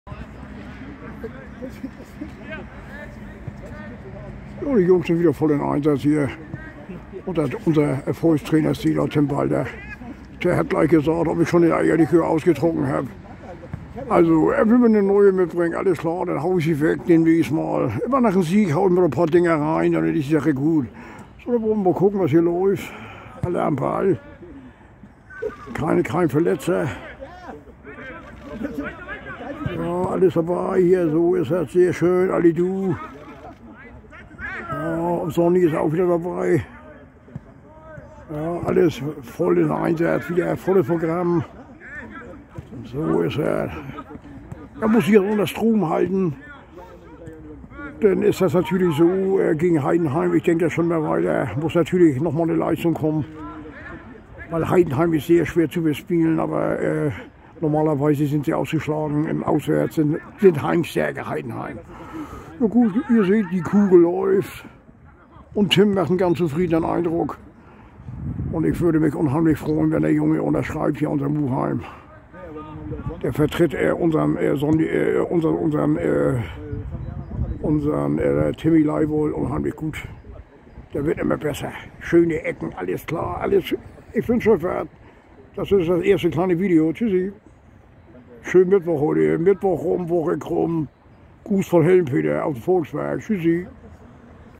Helm-Peter hat sich das erste Training nach den grandiosen Sieg reingepfiffen ! ( Alle motiviert)
Ganz wichtig!! Die Stimmung ist im Volkspark nach 3 Siegen hervorragend & Tim und sein Team kann in Ruhe arbeiten!! NUR DER HSV!!